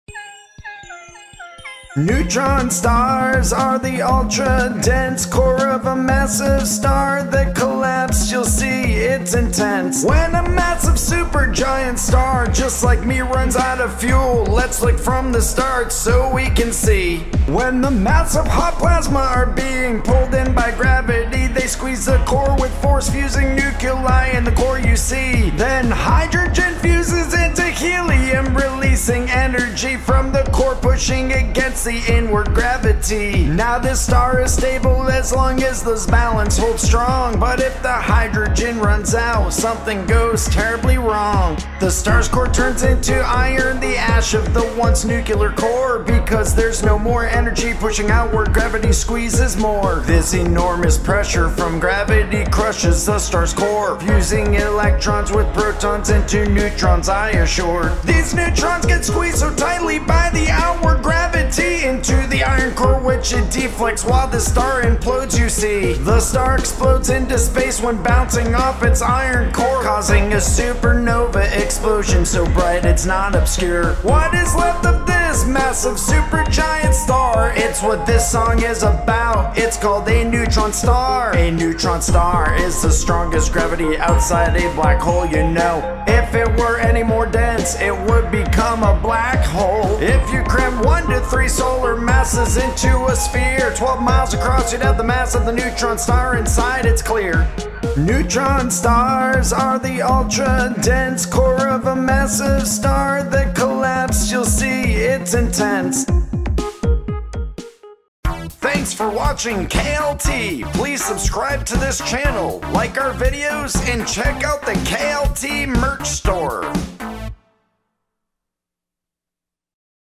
How Neutron Stars Are Born! | KLT
The death of massive super giant stars creates neutron stars, let's learn how that happens!